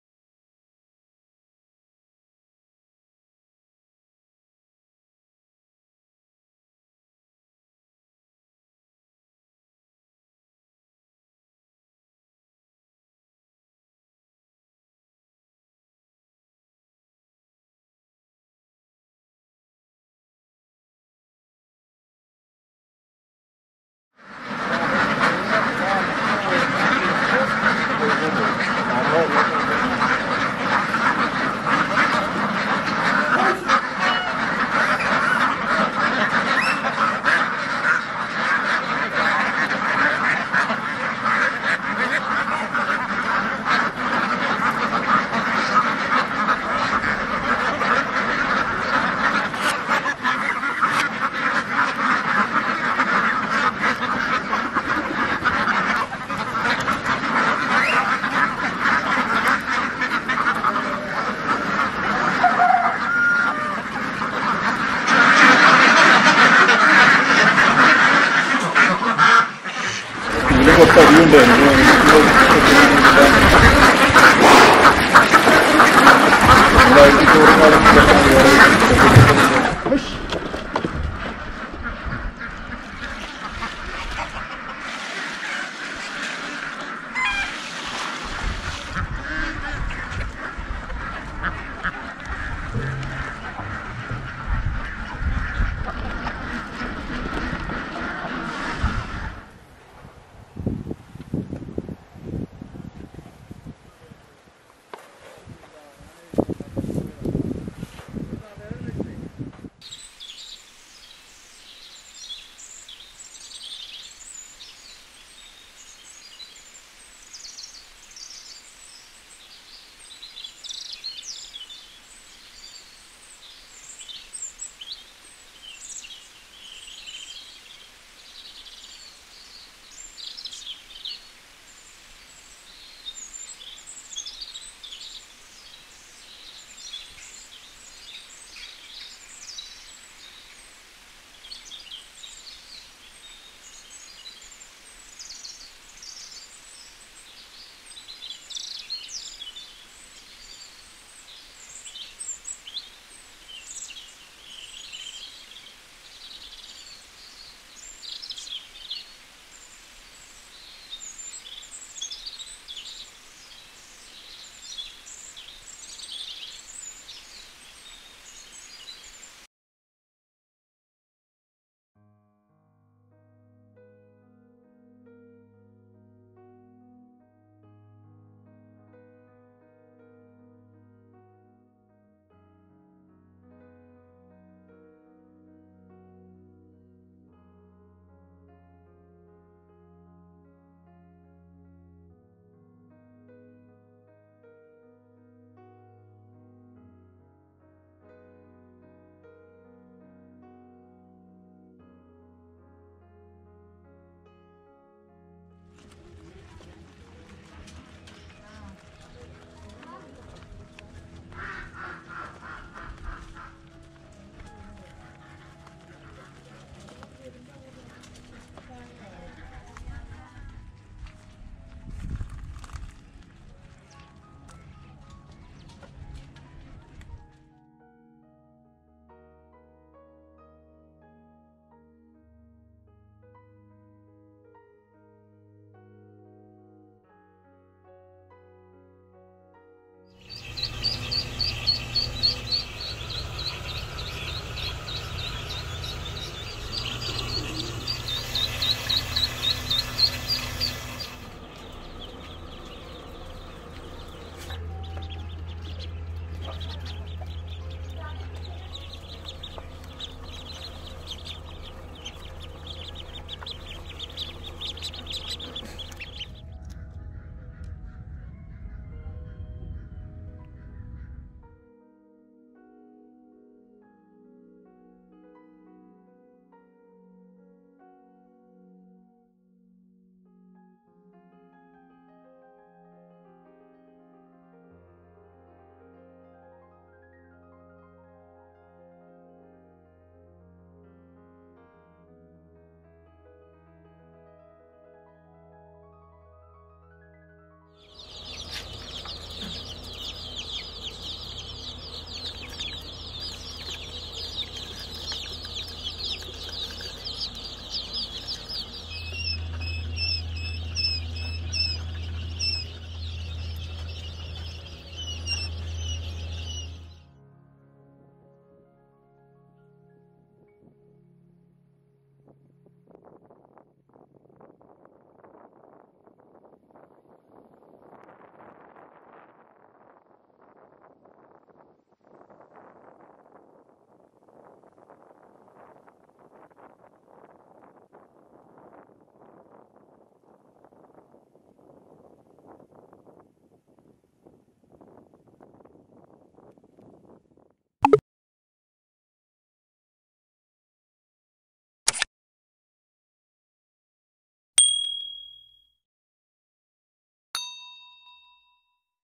CHICKEN LAYS EGG IN DARUSSALAM FARM - KHAMIS MUSHAIT
CHICKEN LAYS EGG IN DARUSSALAM FARM

cyberasia.in

cyberasia